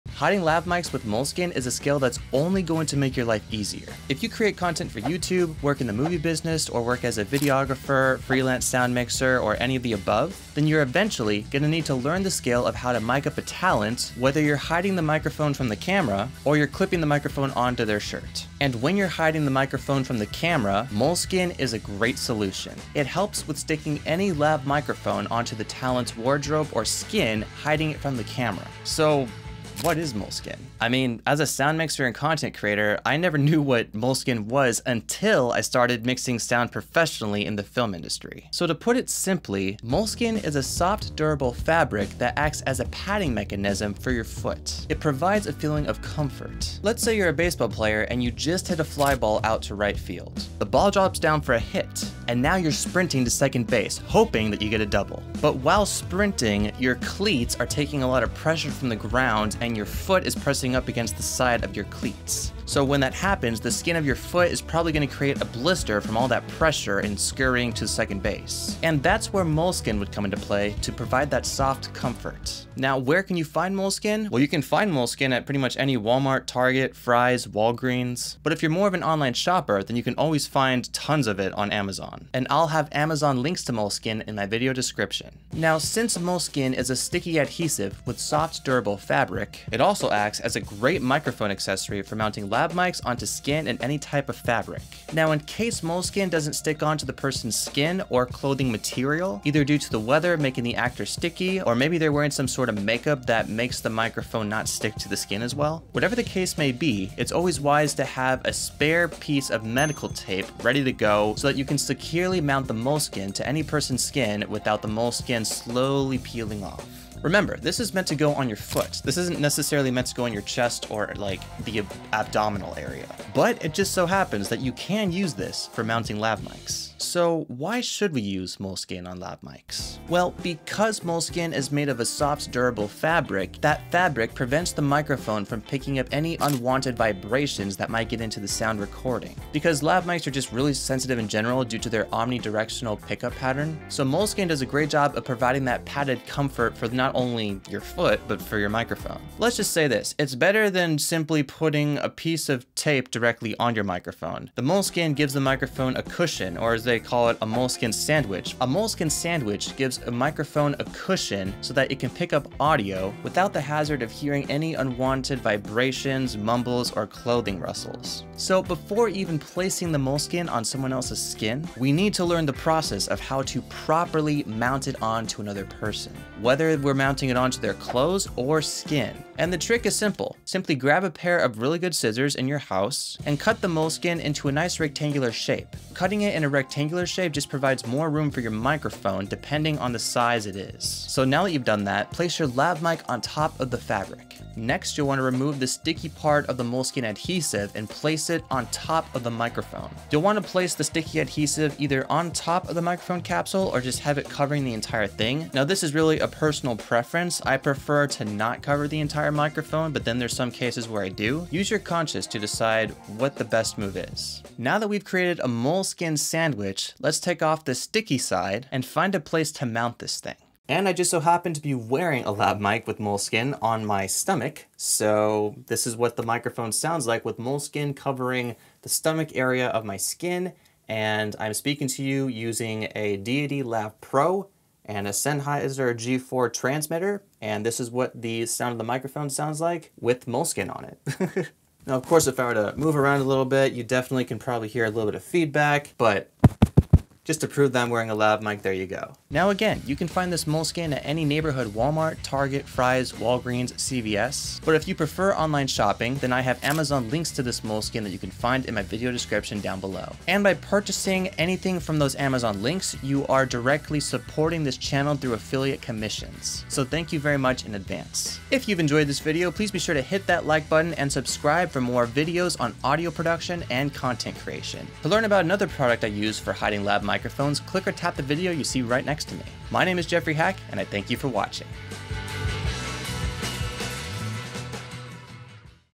This Lav Mic Accessory is What EVERY Creator Needs!
An educational video explaining how to use moleskin (Amazon) with any lav microphone. Also covering what moleskin is and what it's original use is for. This video provides examples of how to create a moleskin sandwich, a key ingredient for mounting lav microphones on skin, shirts, costumes, and other types of fabric.

Moleskin can be used as a microphone accessory for mounting lav mics onto talent's shirts, skin, costumes, and other fabrics, hiding the microphone from the camera.

GET MOLESKIN FOR YOUR NEXT GIG
Doctor Scholl's Moleskin Padding (Amazon) (Amazon)
Moleskin Roll (Amazon) (Amazon)
35 Moleskin Sheets (Amazon) (Amazon)

0:00 Opener
0:28 What is moleskin?
1:12 Where can you find moleskin?
1:25 How moleskin works with microphones
2:07 Why should we use moleskin on lav mic?
2:48 How to make a moleskin sandwich
3:40 Audio test with moleskin on
4:09 Where to purchase moleskine